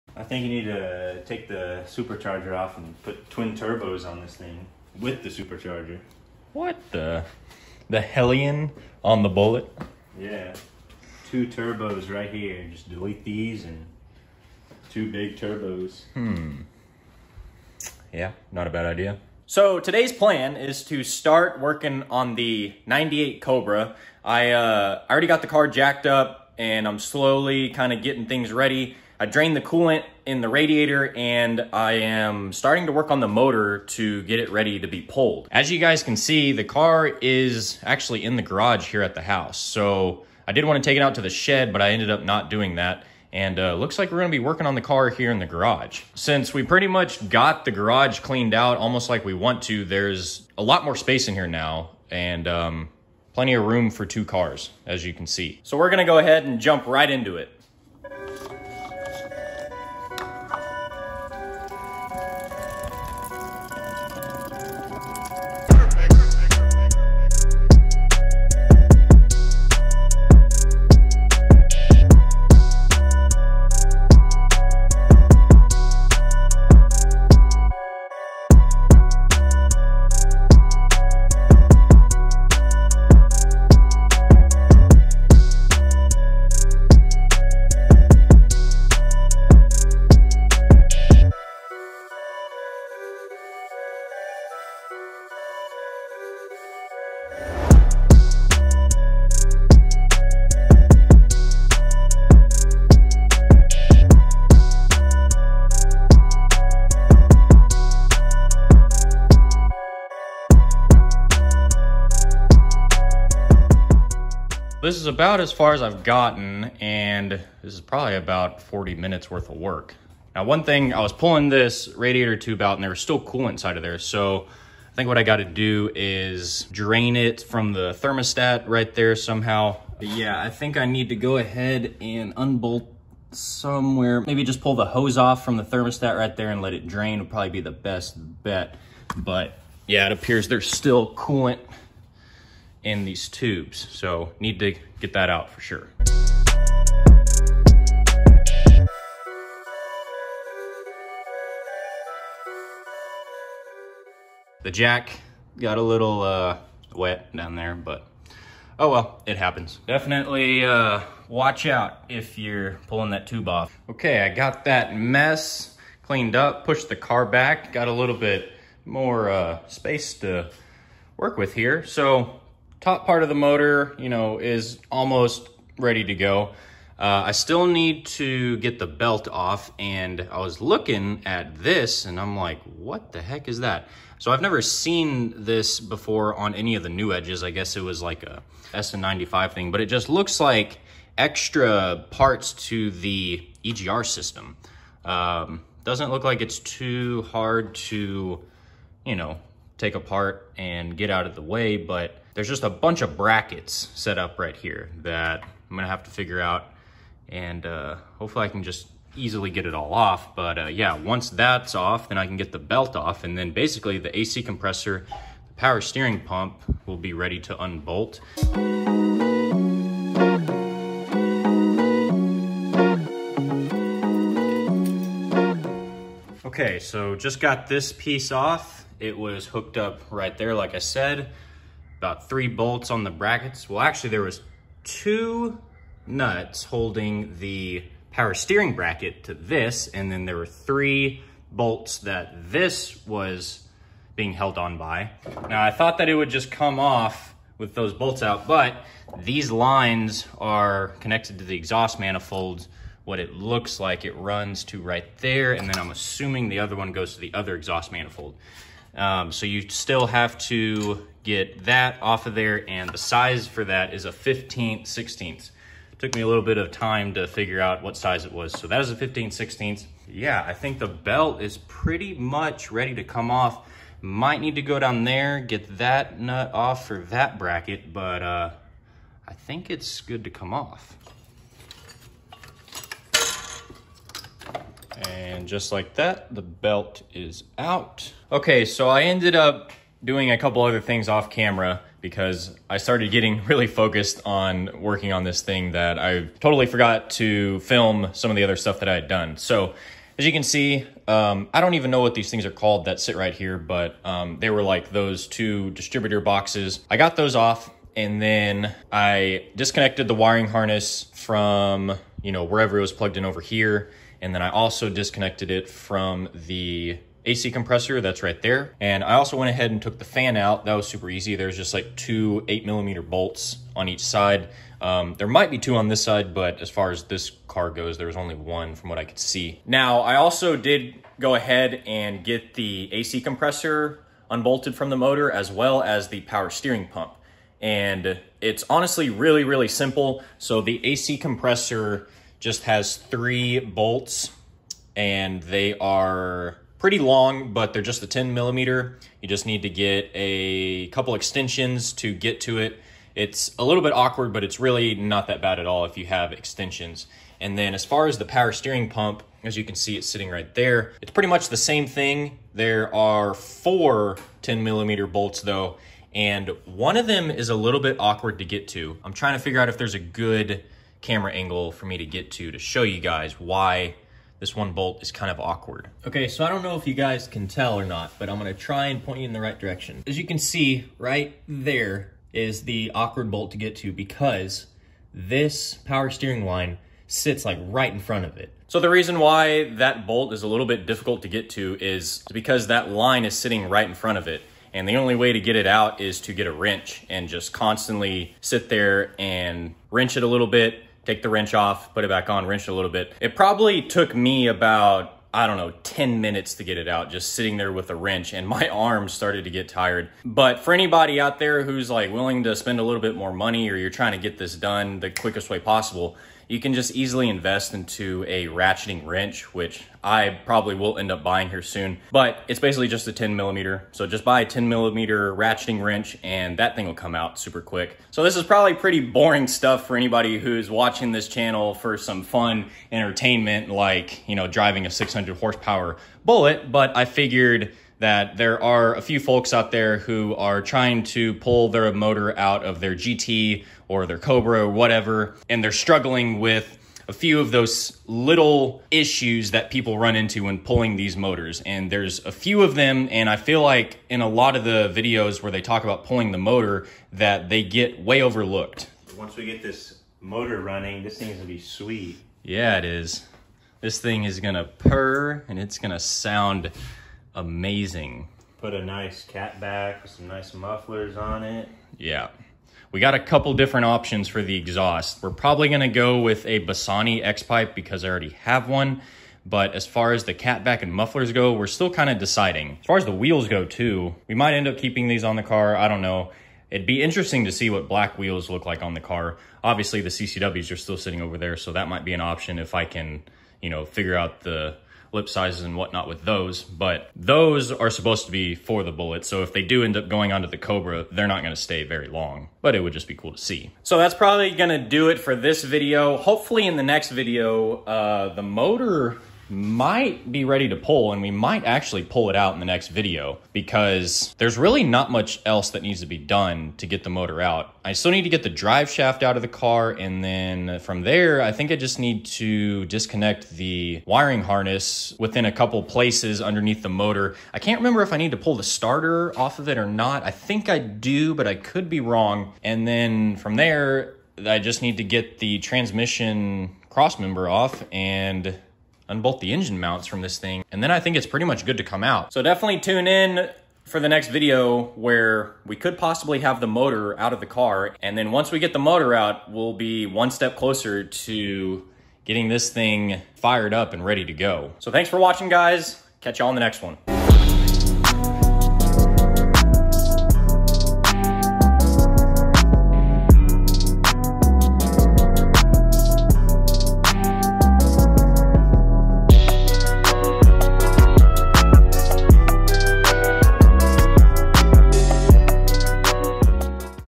Pulling the 4.6 4V ENGINE in the 98 COBRA: PART 1
We have officially started the process of pulling the engine in the 98 cobra. Hopefully it will be all the way out by the end of the month! Enjoy!